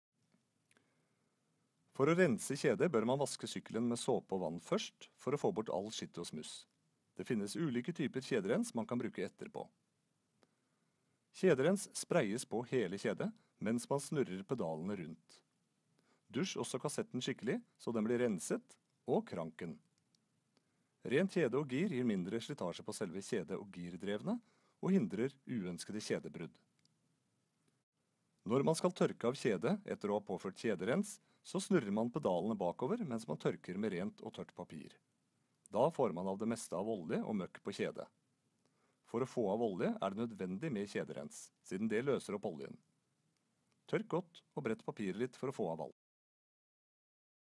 Hvordan ta vare på sykkelen | Rensing av kjedet | XXL
Se våre beste tips for å justere girvaieren på din sykkel.
Trenger du hjelp med sykkelen? Stikk innom din lokale XXL-butikk så hjelper vi deg!

Trenger du nytt sykkelutstyr?